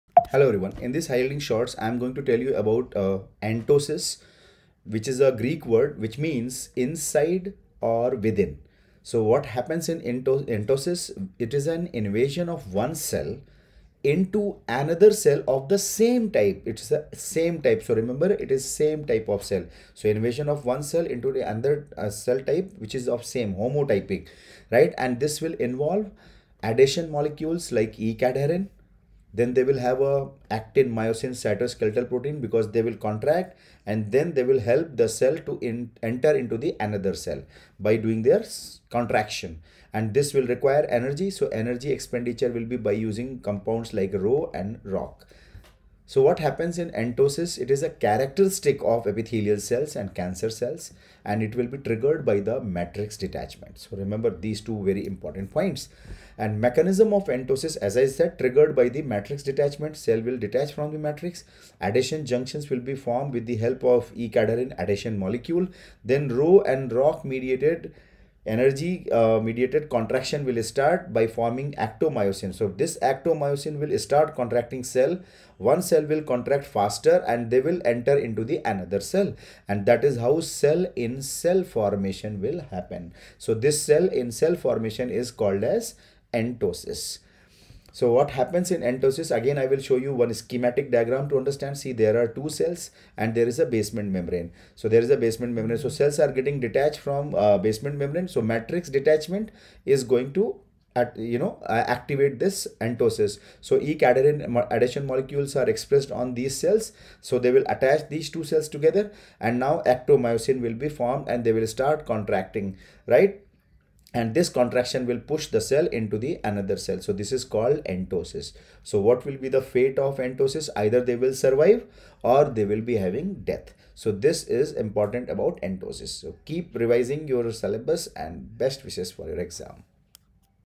High Yielding Shorts-114 (HYS-114):Entosis by Dr Devesh Mishra.
This High Yielding Shorts (HYS) is about Entosis which was question in INICET May 2022.